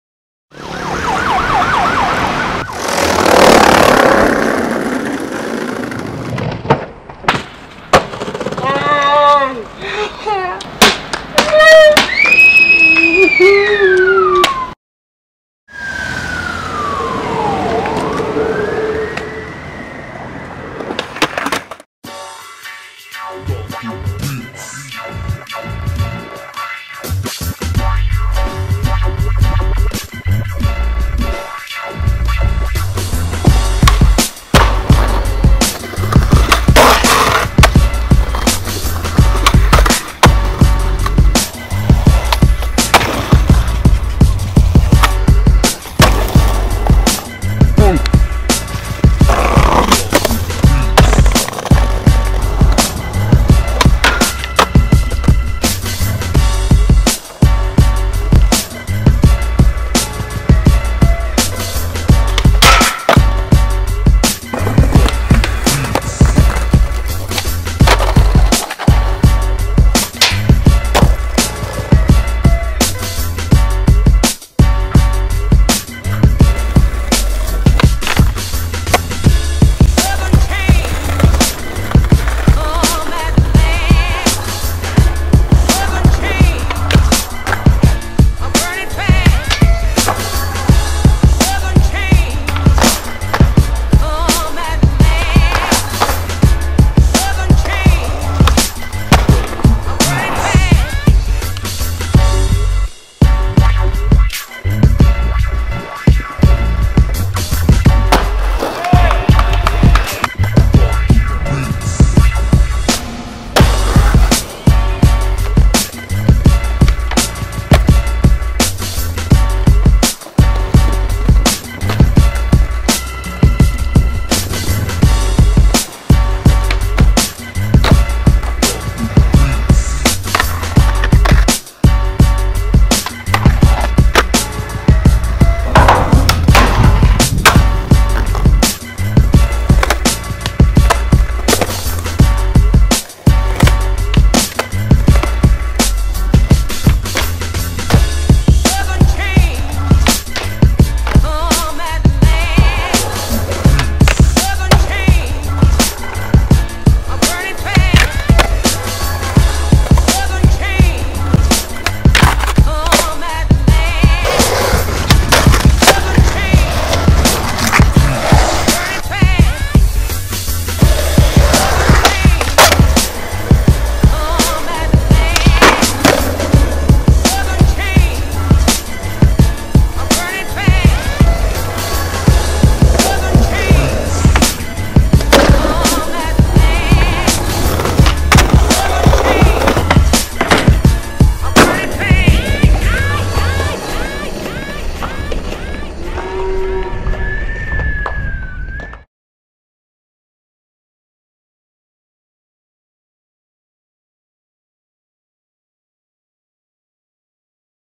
Sessão de Rua #21
Confiram o sessão de rua 21, uma miscelânea de imagens por toda parte e em várias cidades.